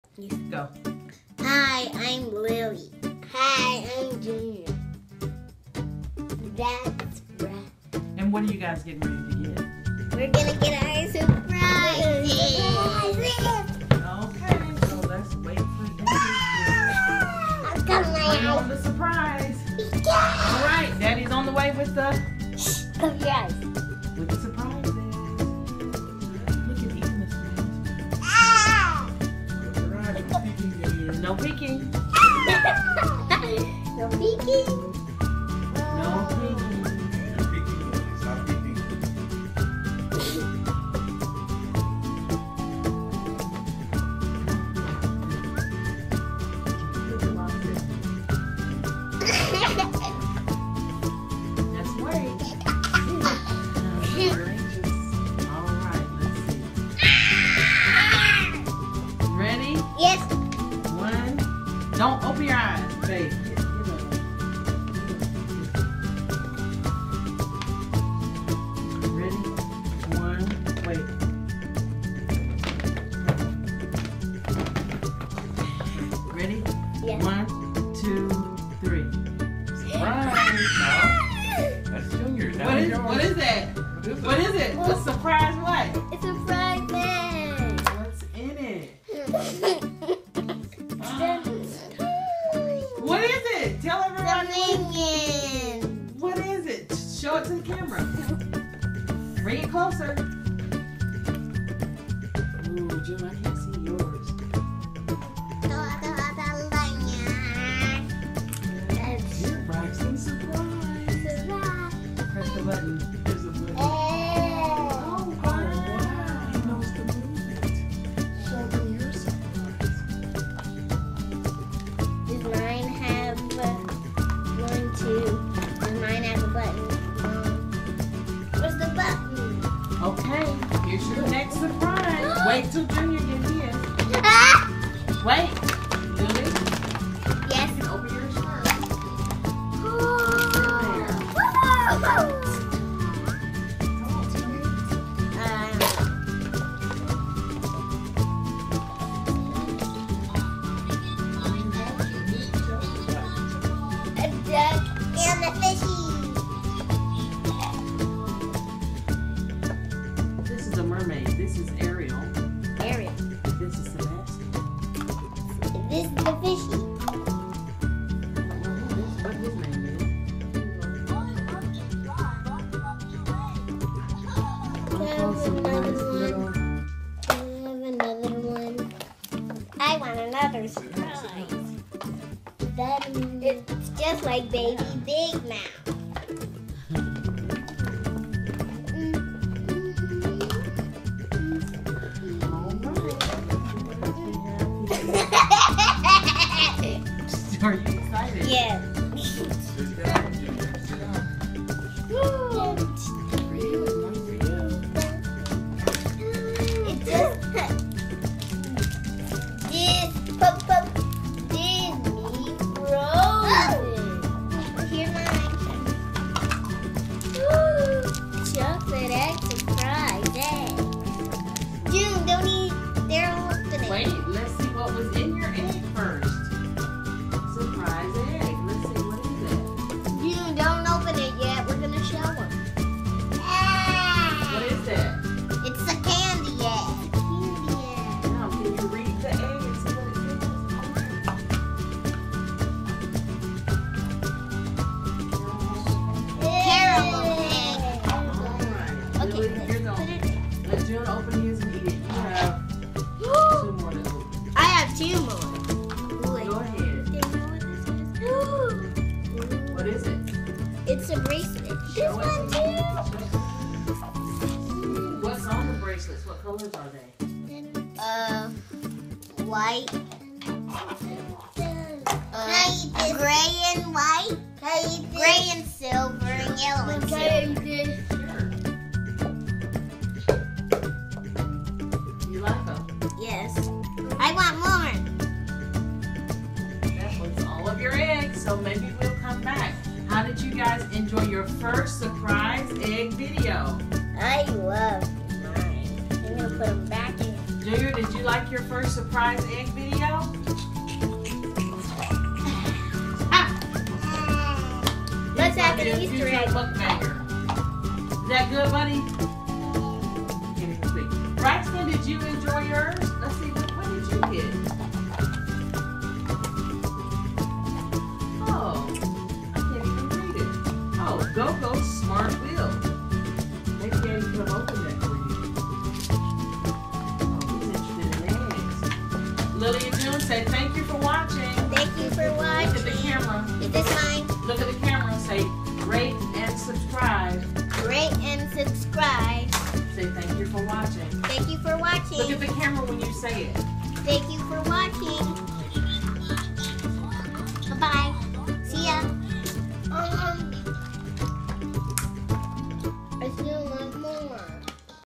Their First Surprise Eggs!!
Here is our very first Surprise Egg Video! My kids watch a lot of these kind of videos so I thought I would try this! We will be doing lots more of these because no only do they love getting surprise toys, I love recording their reactions to the little things! Enjoy!